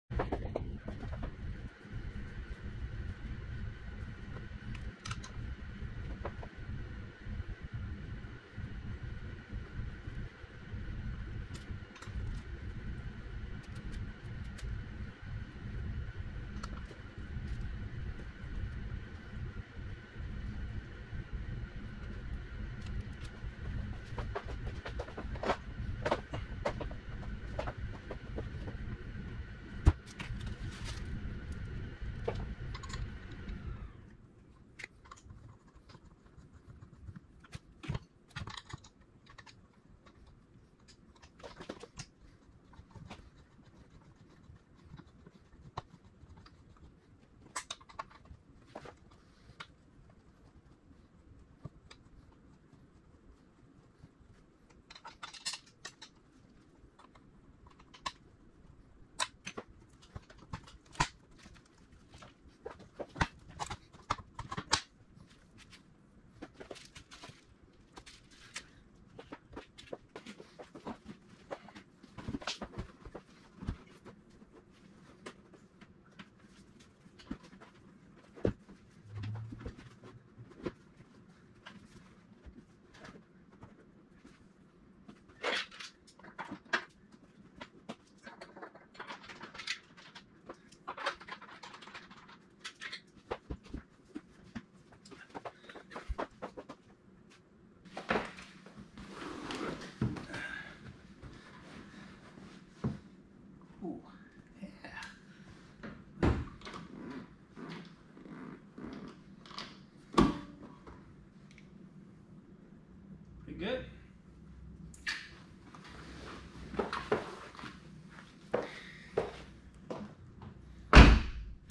HowTo: Corbeau A4 Racing Seat Install in a 2005 Nissan 350Z
This video demonstrates the steps to install a Corbeau A4 Racing Seat on the passenger side of a Nissan 350Z. These weigh less than the OEM leather seats, are comfortable, and hold you in place during tight cornering!

This presentation is protected by the U.S. and international copyright laws.  Reproduction and distribution in any form without the written permission of Rob Review is prohibited.